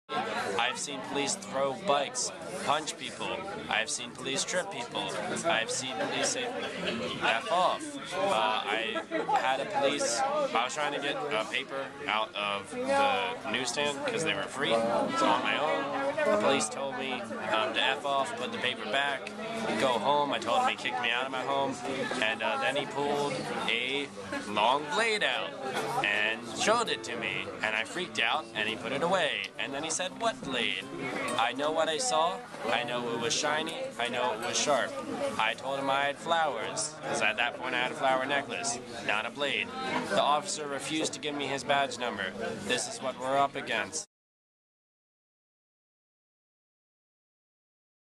Police accused of pulling a knife on protester at Occupy Philly eviction
In the early morning hours of Nov. 30 Occupy Philly was evicted by hundreds of police officers. Protesters marched through the streets for hours while police followed, surrounded, and at times violently attacked peaceful protesters. Around 4:30 am police arrested about 50 people. Shortly afterwards protesters went to the American Friends Services Committee building and then to Thomas Paine Plaza to share some of their stories.